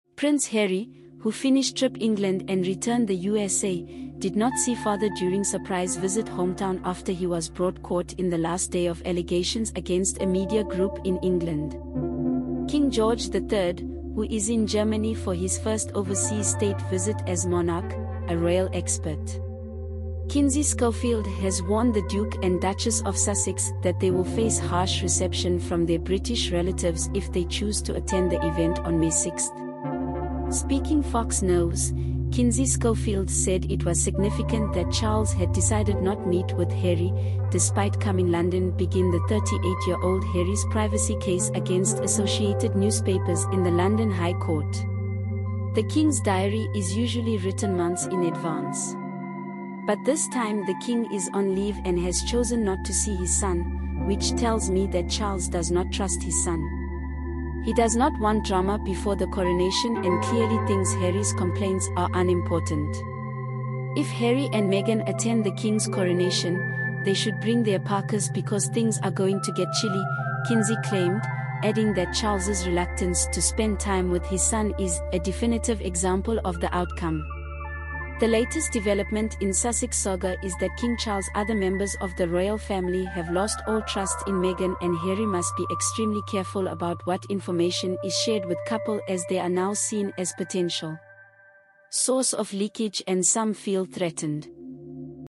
King Charles willingly avoids Prince Harry as royal family loses all trust in Sussexes?
Prince Harry, who finished trip England and returned the USA, did not see father during surprise visit hometown after he was brought court in the last day of allegations against a media group in England. King George III, who is in Germany for his first overseas state visit as monarch, a royal expert. Kinsey Schofield has warned the Duke and Duchess of Sussex that they will face harsh reception from their British relatives if they choose to attend the event on 6 May. Speaking Fox Nerws, Kinsey Schofield said it was significant that Charles had decided not meet with Harry, despite coming London begin the 38-year-old Harry's privacy case against Associated Newspapers in the London High Court. "The king's diary is usually written months in advance. But this time the king is on leave and has chosen not to see his son, which tells me that Charles does not trust his son. He does not want drama before the coronation and clearly thinks Harry's complaints are unimportant." "If Harry and Meghan attend the king's coronation, they should bring their parkas because things are going to get chilly," Kinsey claimed, adding that Charles' reluctance to spend time with his son is "a definitive example of the outcome." The latest development in Sussex saga is that King Charles other members of the royal family have lost all trust in Meghan and Harry must be extremely careful about what information is shared with couple as they are now seen as potential. source of leakage and some feel threatened.